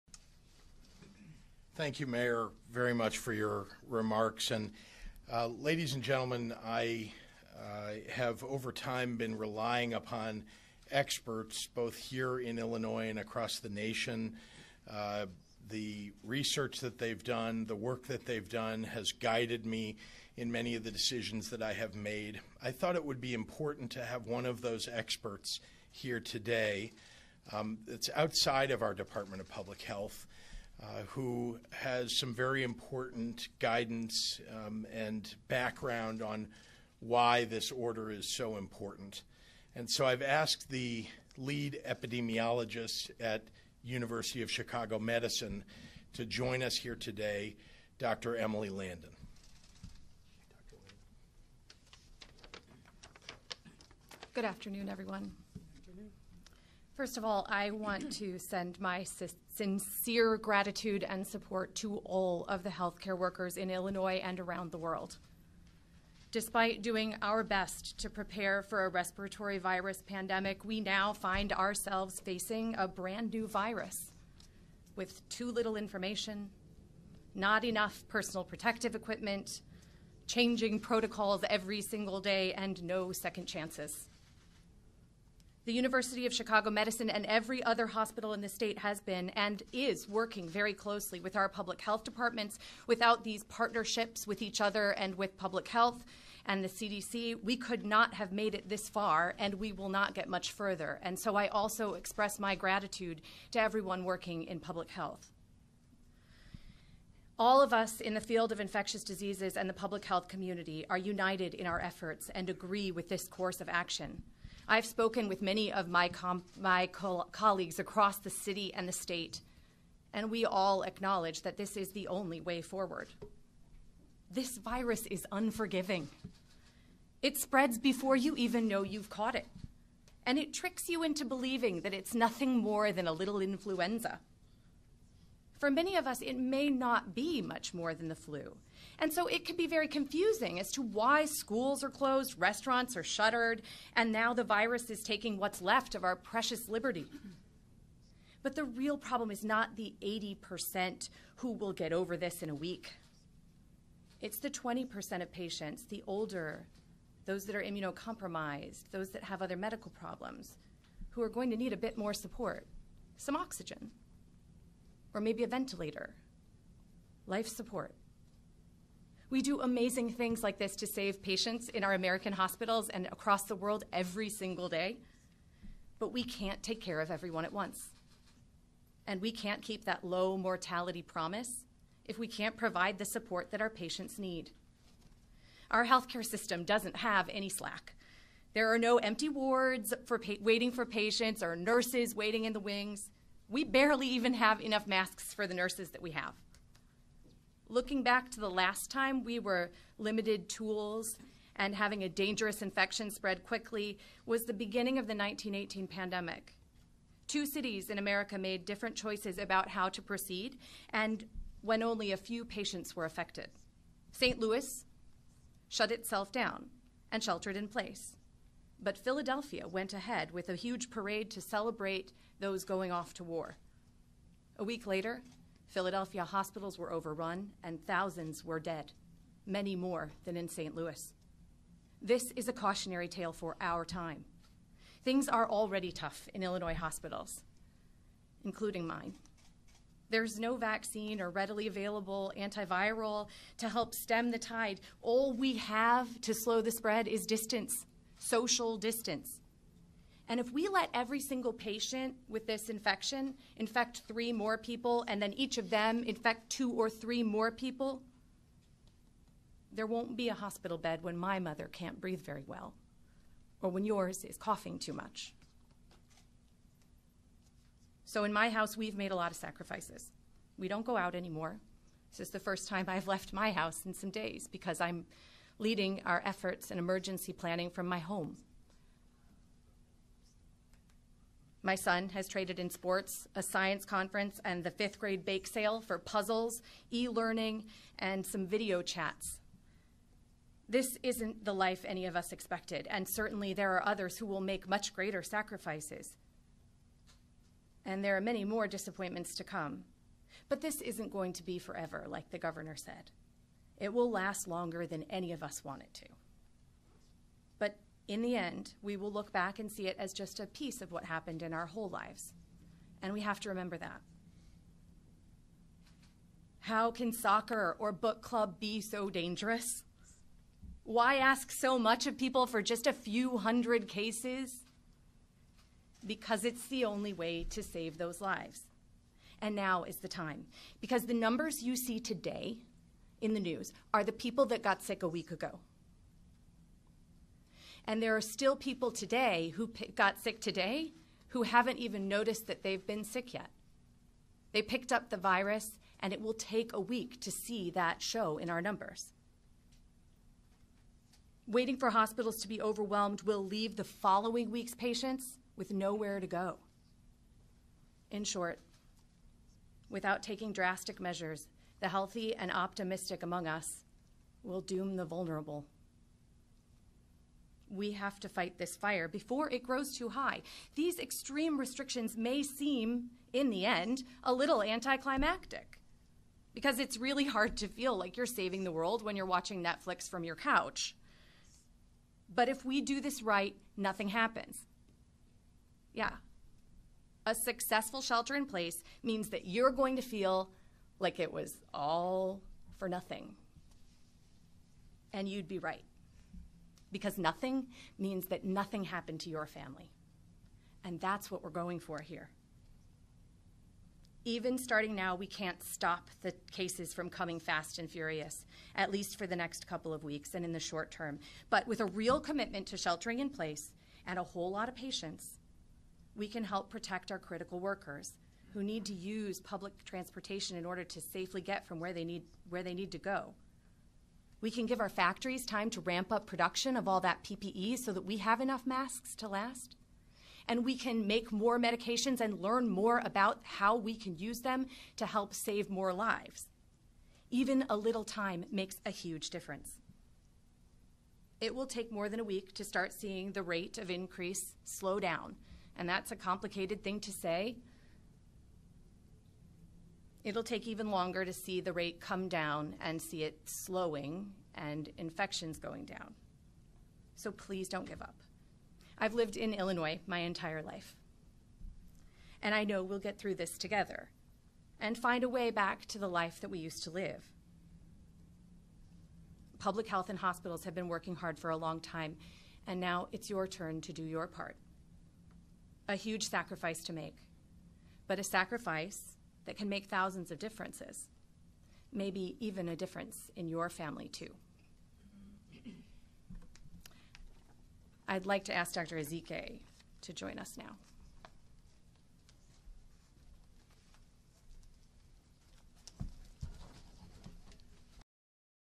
Dr. Landon, lead Epidemiologist at University of Chicago Medicine speakes to Illinois about Covid-19
Our Governor, JB Pritzker yesterday, just ordered that all non-essential workers in Illinois must stay at home and shelter in place in the fight of this Coronavirus. He said that over the last few weeks he had reached out to and relied upon some of the best medical experts, epidemiologists, mathematicians and modelers to help him understand what the progression of this disease will look like in Illinois. After he was done he asked Dr. Emily Landon, the lead Epidemiologist at University of Chicago Medicine to speak. It's only a little over 8 minutes, but it speaks volumes of what we are ALL doing this for.

This is her amazing speech

I do not own this and am displaying this for the sake of public health information